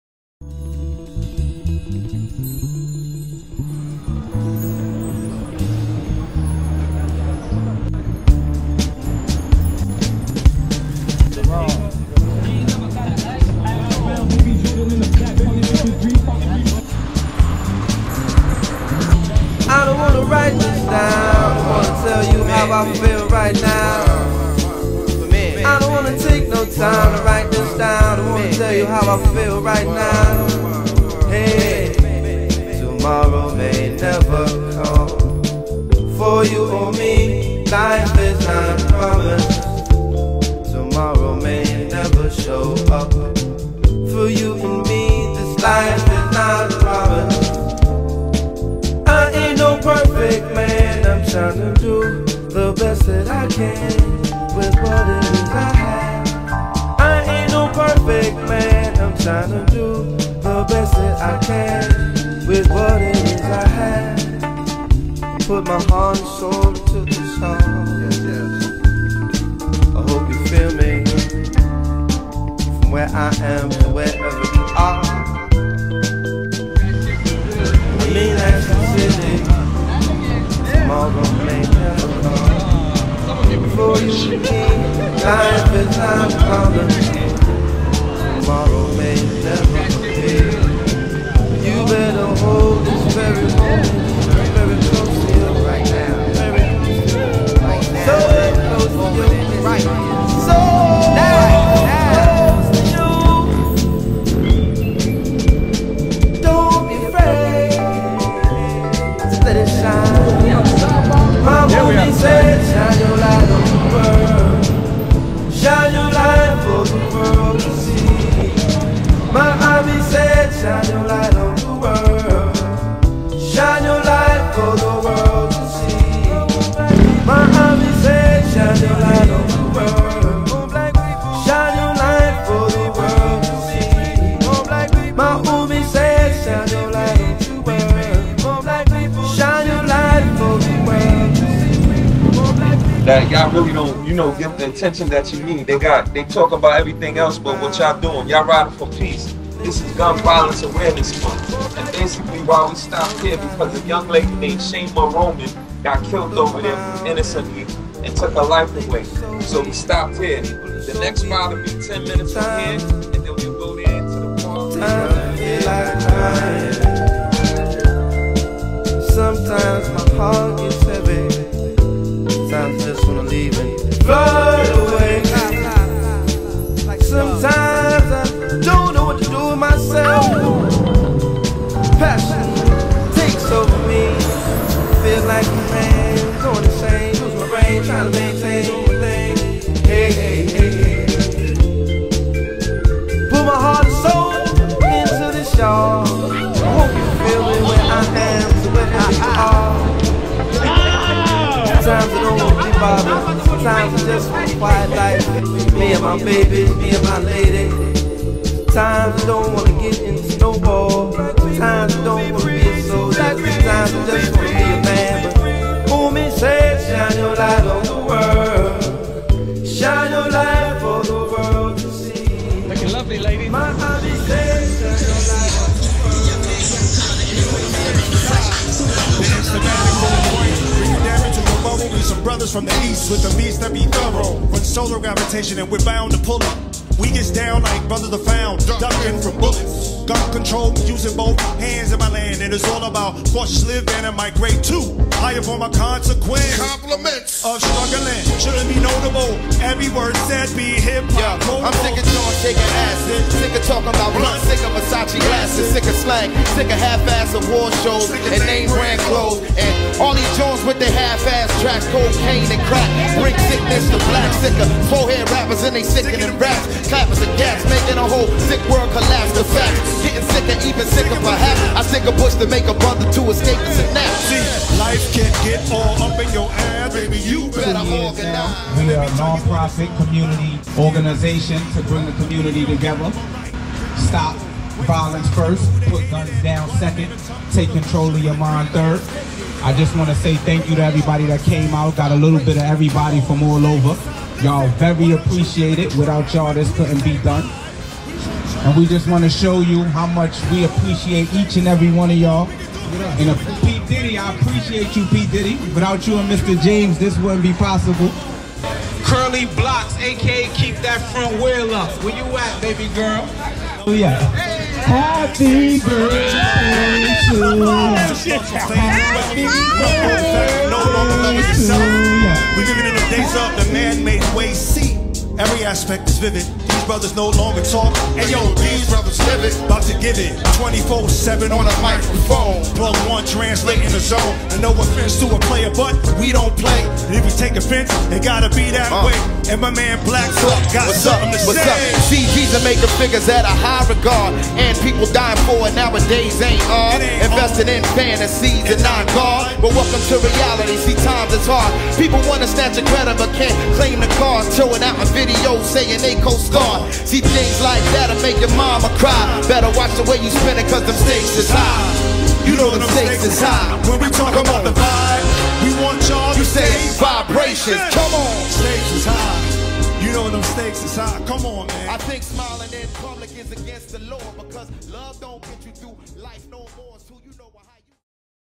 CHOOSE THE RIGHT GRIP - Gun Violence Awareness Rideout (Extended version)
Brooklyn NY - June 4th 2023

Elite Learners along with James Mooney arranged this very special event to raise awareness of gun violence in our city amongst the riding community of NYC.

Credits
Ian Bartlett